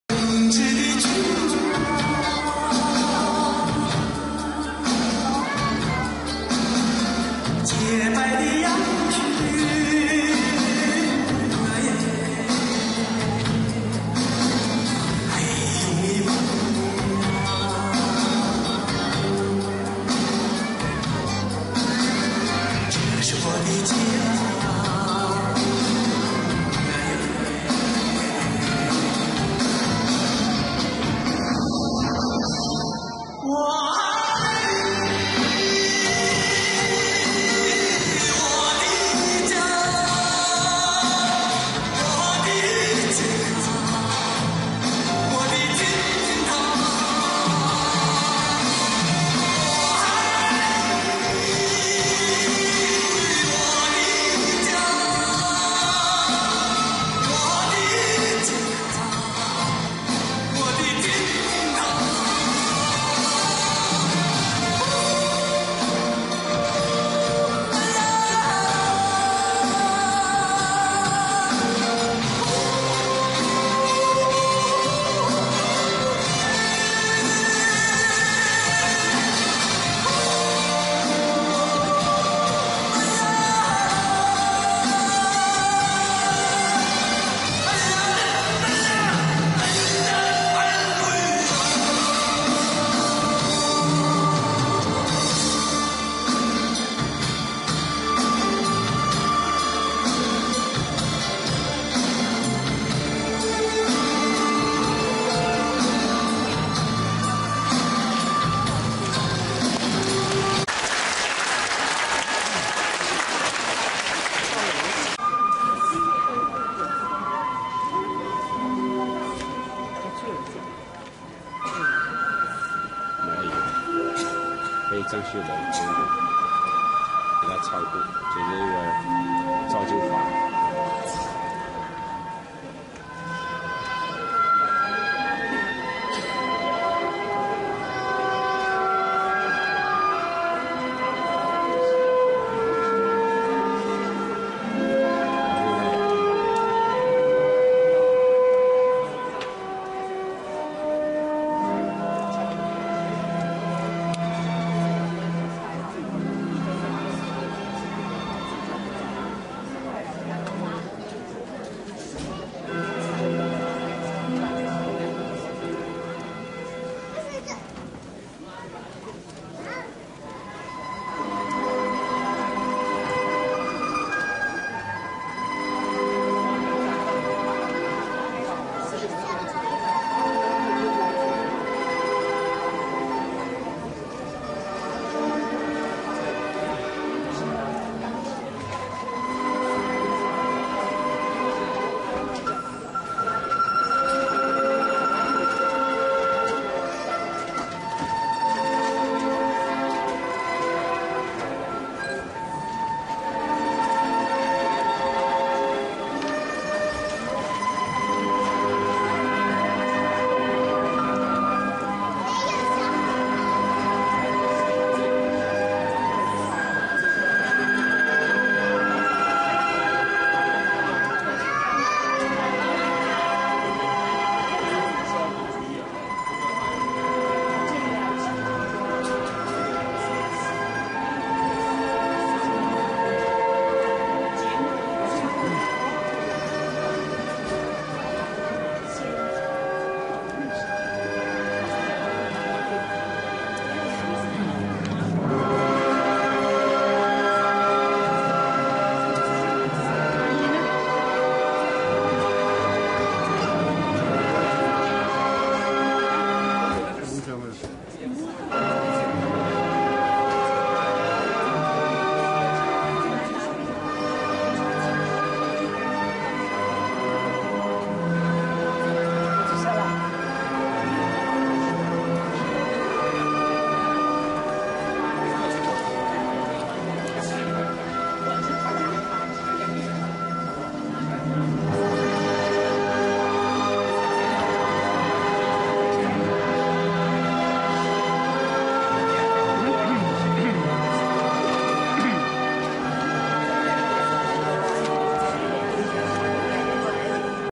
Olivia Chow Chinese New Year Celebration 2012 - Part 3
Chinese New Year 2012: Olivia hosted a public celebration with multicultural music and dance program, showcasing the diversity of Toronto's Chinatown and Downtown community.